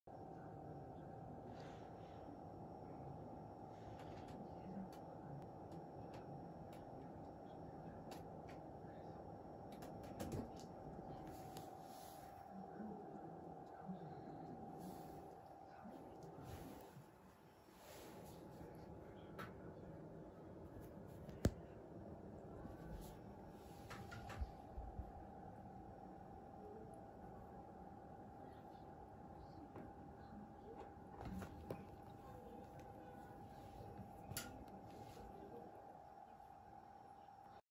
mochas first night at the house
lmao my rooms so messy it’s only cuz the cat just got here ;)